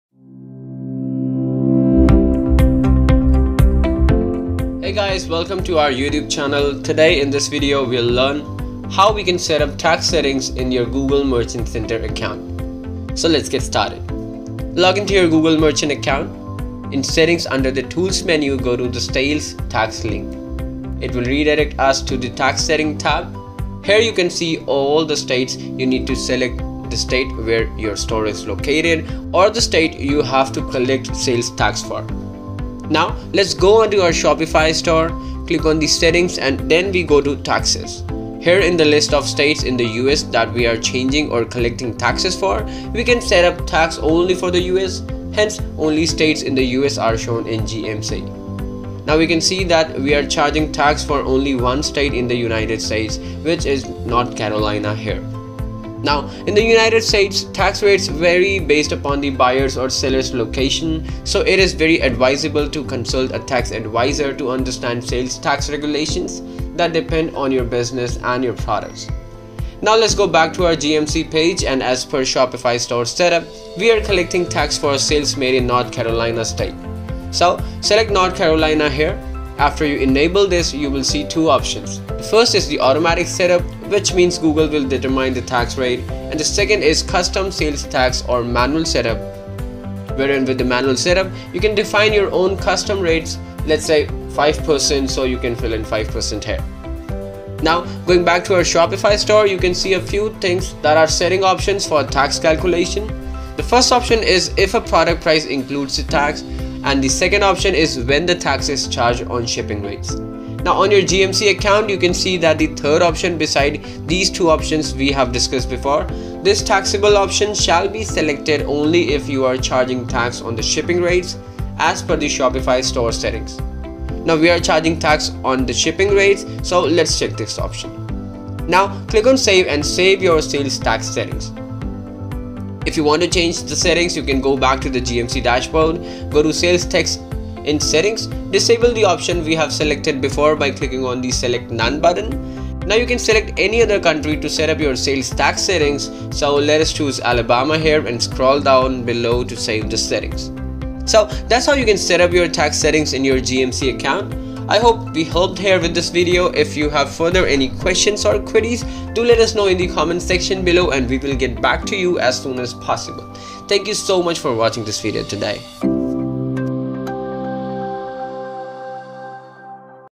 How To Set Up Tax Settings in Google Merchant Center
If you are selling within the United States and want to know how to set up tax settings in your merchant account, this video is for you!

If you are an online seller based in the United States, it is compulsory to set up tax rates for each state in the U.S. that you charge taxes for.

If you are selling outside the USA, the taxes are included in the product price, and hence you just need to confirm that you are not selling in the USA. Google will automatically know that you are not required to collect taxes.

Tax rates vary depending on the location of your customers or the products you are selling.

You get two options to choose from to determine tax in each state.

1. Custom Sales Tax - where you can enter a flat rate if you charge the same tax rate for all the customers regardless of where they are located.

2. Google Determined Sales Tax - This is the automatic setup in which you allow Google to determine tax rates depending on the location of your customers.

Setting up taxes in GMC account can be a bit tricky, so it is advisable to consult a professional tax advisor to know what would work best for you.

🔗 Link to Install our Suite For Google Shopping Feed App By CedCommerce